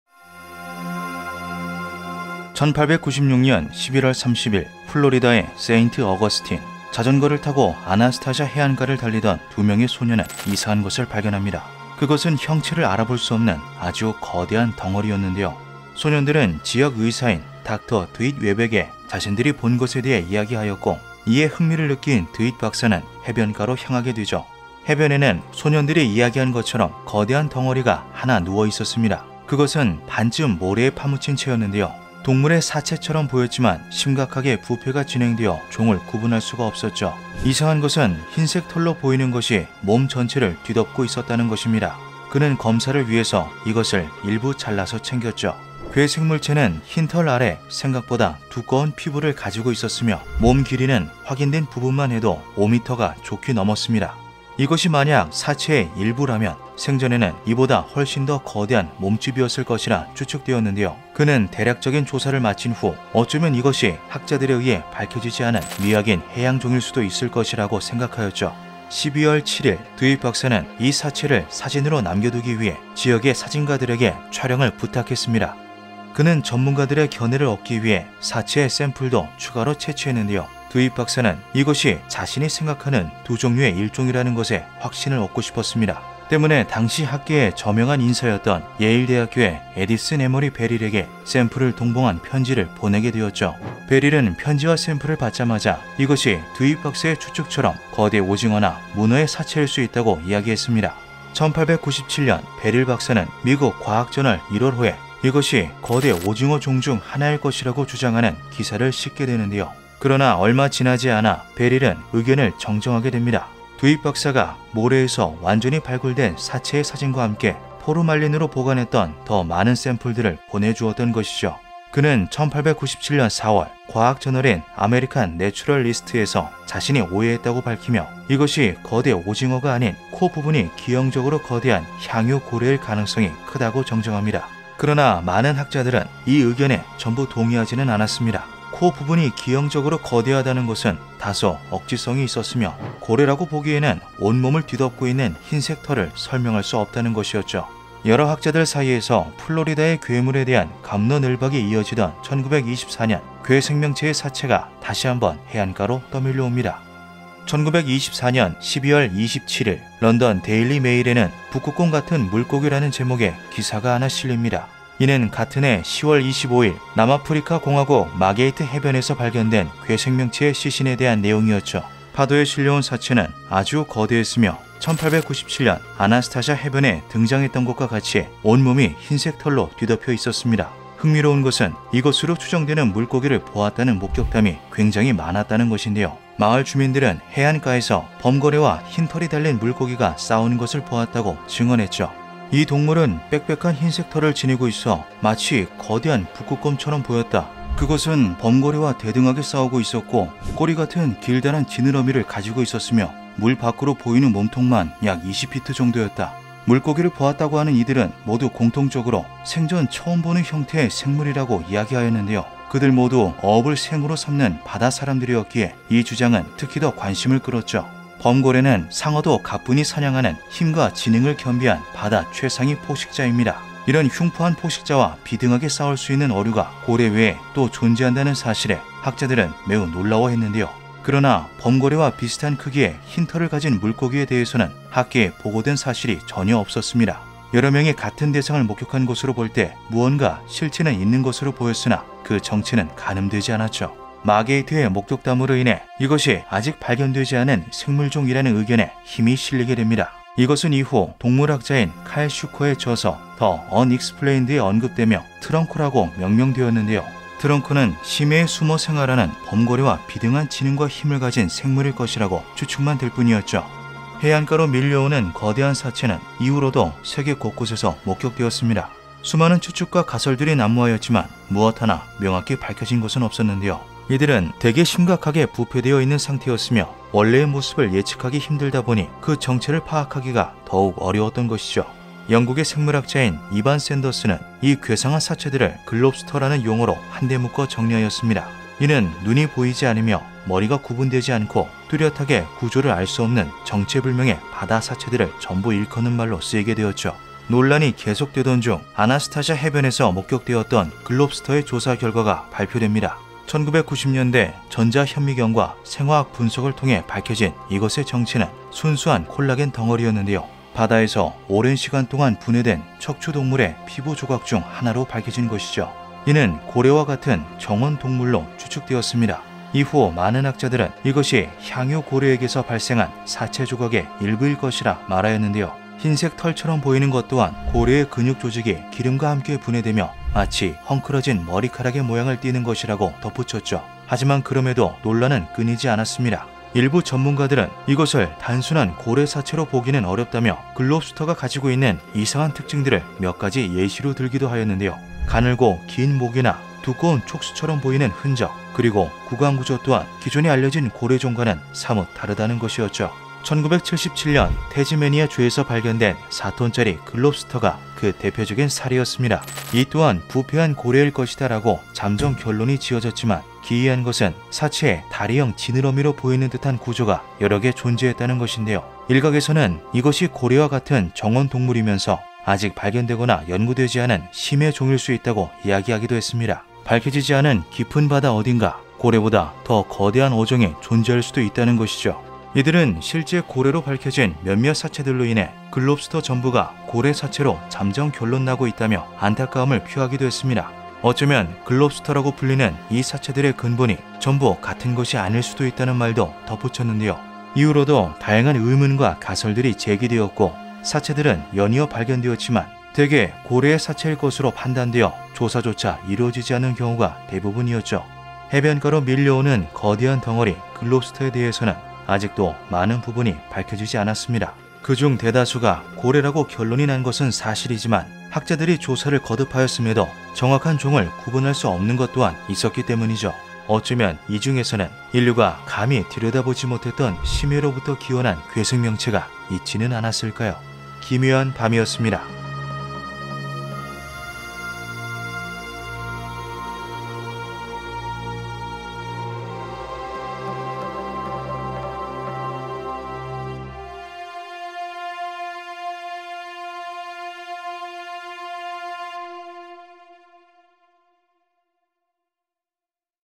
1896년부터 전세계 곳곳의 해안가에서 발견되기 시작한 정체불명의 심해 물고기 [미스터리]
과연, 글롭스터의 진짜 정체는 무엇일까요?